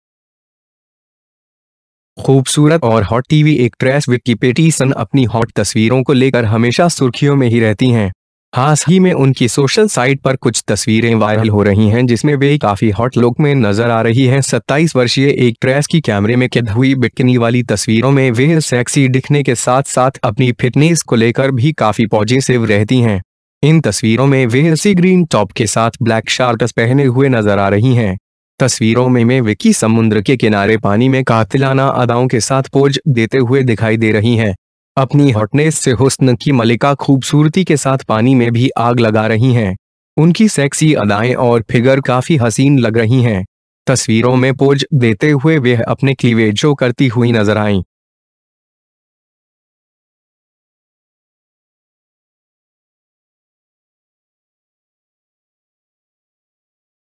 तस्वीरो में देखे अपनी फिटनेस को लेकर कितनी पॉजेसिव है यह एक्ट्रेस
खूबसूरत और हॉट टीवी एक्ट्रैस विक्की पेटीसन अपनी हॉट तस्वीरों को लेकर हमेशा सुर्खियों में ही रहती हैं। हास ही में उनकी सोशल साइट पर कुछ तस्वीरें वायरल हो रही हैं जिसमें वह काफी हॉट लुक में नजर आ रही हैं।
27 वर्षीय एक्ट्रैस की कैमरे में कैद हुई बिकिनी वाली तस्वीरों में वह सैक्सी दिखने के साथ-साथ अपनी फिटनेस को लेकर भी काफी पॉजेसिव रहती हैं।
इन तस्वीरों में वह सी-ग्रीन टॉप के साथ ब्लैक शार्टस पहने हुए नजर आ रही हैं। तस्वीरों में में विक्की समुंद्र के किनारे पानी में कातिलाना अदाओं के साथ पोज देते हुए दिखाई दे रही हैं।
अपनी हॉटनेस से हुस्न की मलिका खूबसूरती के साथ पानी में भी आग लगा रही हैं। उनकी सैक्सी अदाएं और फिगर काफी हसीन लग रही हैं। तस्वीरों में पोज देते हुए वह अपने क्लीवेज शो करती हुईं नजर आईं।